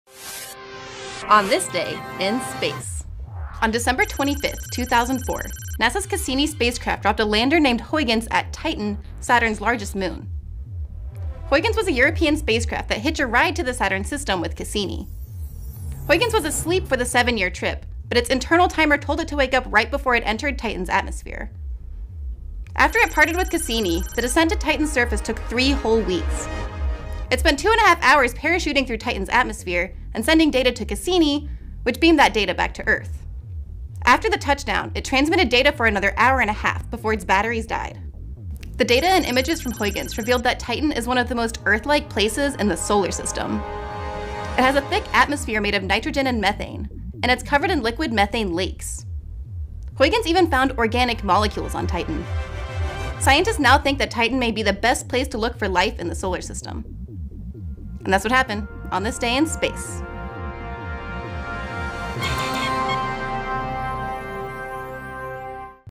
Cassini deployed probe above Titan - On This Day in Space | Dec. 25
On Dec. 25, 2004, NASA’s Cassini spacecraft dropped a lander named Huygens at Titan, Saturn’s largest moon. -- ‘On This Day in Space’ Video Series on Space.com

Huygens was a European spacecraft that hitched a ride to the Saturn system with Cassini. Huygens was asleep for the seven-year trip, but its internal timer told it to wake up right before it entered Titan’s atmosphere.

After it parted with Cassini, the descent to Titan’s surface took three whole weeks. It spent two and a half hours parachuting through Titan’s atmosphere and sending data to Cassini, which beamed that data back to Earth.

After the touchdown, it transmitted data for another hour and a half before its batteries died. The data and images from Huygens revealed that Titan is one of the most Earth-like places in the solar system. It has a thick atmosphere made of nitrogen and methane, and it's covered in liquid methane lakes. Huygens even found organic molecules on Titan.

Scientists now think Titan may be the best place to look for life in the solar system.